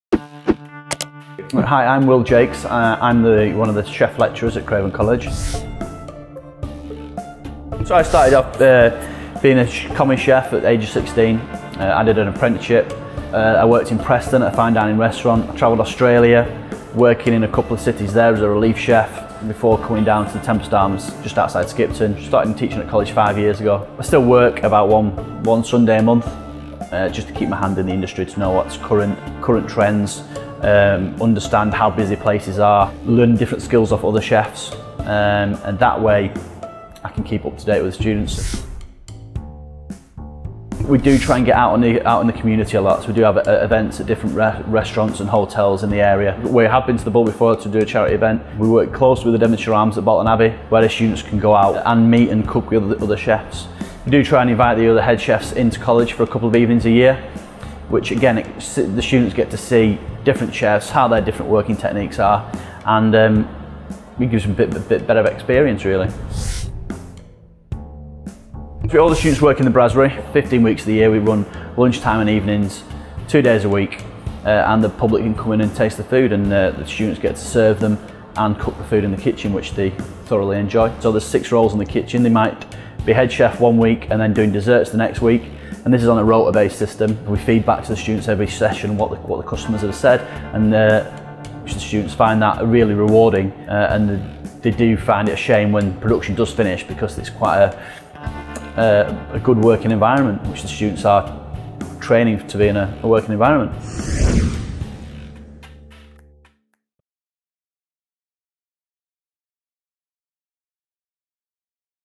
Catering & Hospitality - Will Jacques
Chef and lecturer Will Jacques talks about his background in the  catering industry and how that effects his teaching.